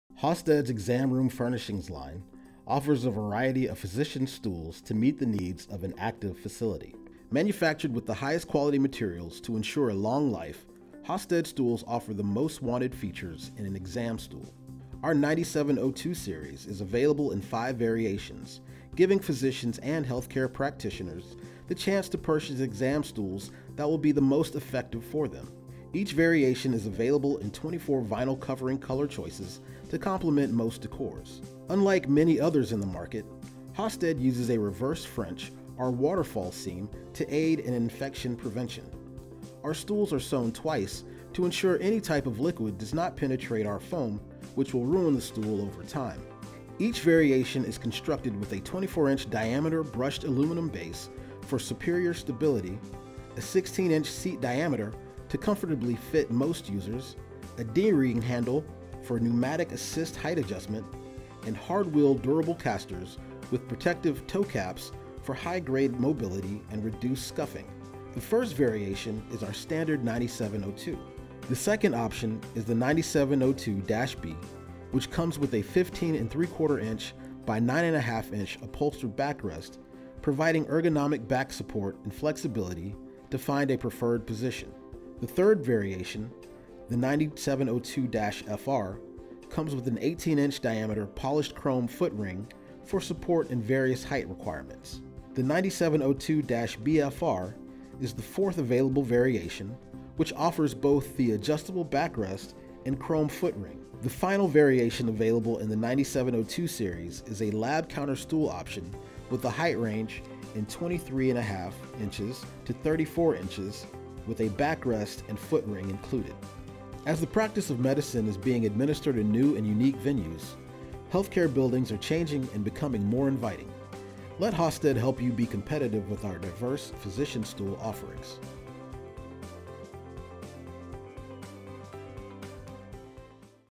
Hausted® Physician Exam Room Stool 9702 Series
Hausted's Exam Room Furnishings line offers a variety of medical stools to meet the needs of an active facility. Manufactured with the highest quality materials to ensure a long life, Hausted stools offer the most wanted features in an exam stool.

Our 9702 Series is available in 5 variations, giving physicians and healthcare practitioners the chance to purchase exam stools that will be the most effective for them. Each variation is available in 24 vinyl covering color choices to complement most decors.

Unlike many others in the market, Hausted uses a reverse French or waterfall seam to aid in infection prevention. Our stools are sewn twice to ensure any type of liquid does not penetrate our foam which will ruin the stool over time.

Each variation is constructed with a 24-inch diameter brushed aluminum base for superior stability, a 16-inch seat diameter to comfortably fit most users, a D-ring handle for pneumatic assist height adjustment, and hard wheel durable casters with protective toecaps for high-grade mobility and reduced scuffing.

The first variation is our standard 9702.

The second option is the 9702-B which comes with a 15.75" x 9.5” Upholstered backrest providing ergonomic back support and flexibility to find a preferred position.

The third variation - the 9702-FR -comes with an 18-inch diameter polished chrome foot ring for support in various height requirements.

The 9702-BFR is the fourth available variation which offers both the adjustable backrest and chrome foot ring.

The final variation available in the 9702 series is a lab/counter stool option with a height range is 23.5 to 34 in and a backrest and foot ring included

As the practice of medicine is being administered in new and unique venues, healthcare buildings are changing and becoming more inviting. Let Hausted help you be competitive with our diverse physician stool offering.